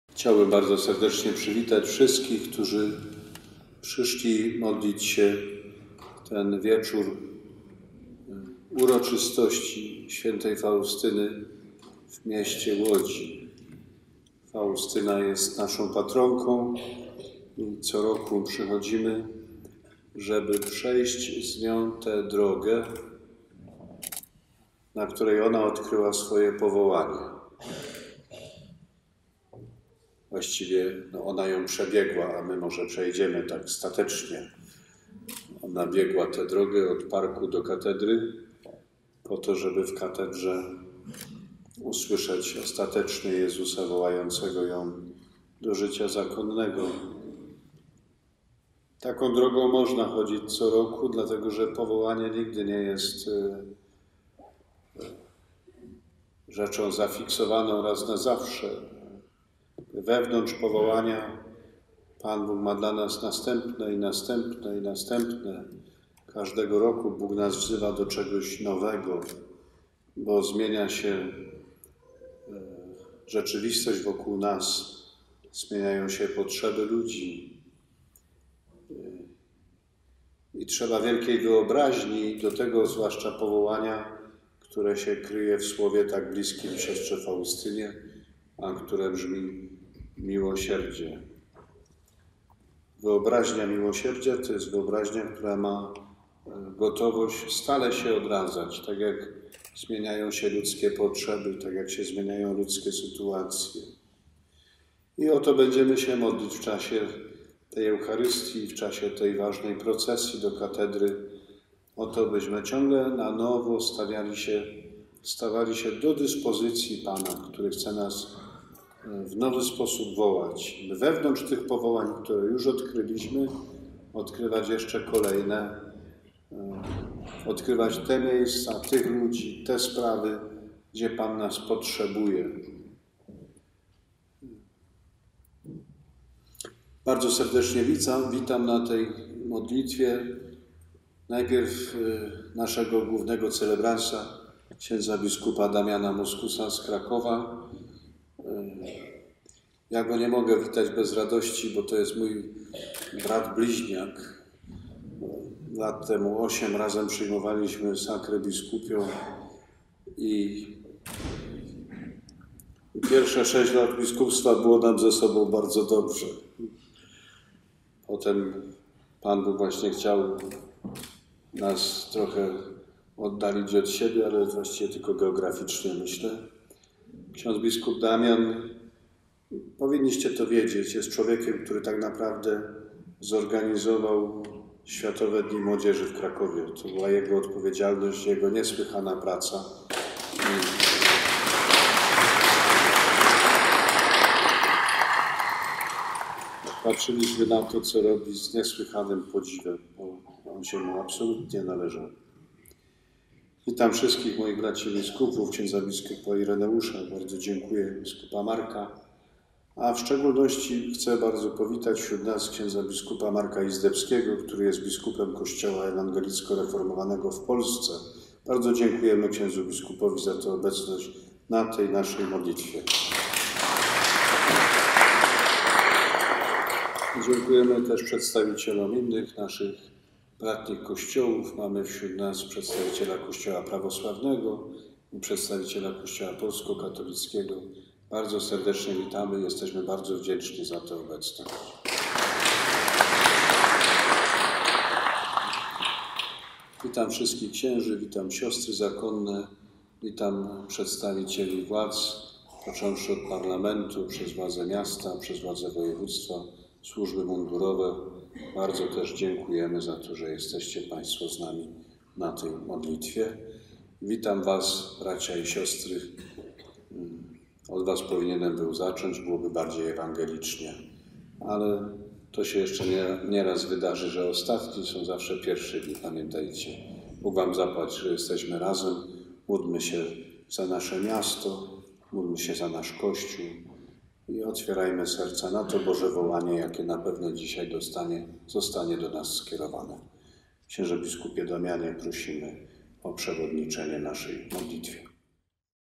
Abp Grzegorz Ryś na uroczystości ku czci św. Faustyny
Abp Grzegorz Ryś powitał 5 października 2019 r. wiernych zebranych na Mszy św. w uroczystość patronki Łodzi, św. Faustyny, w łódzkiej parafii pod jej wezwaniem. Liturgii przewodniczył bp Damian Muskus, biskup pomocniczy archidiecezji krakowskiej.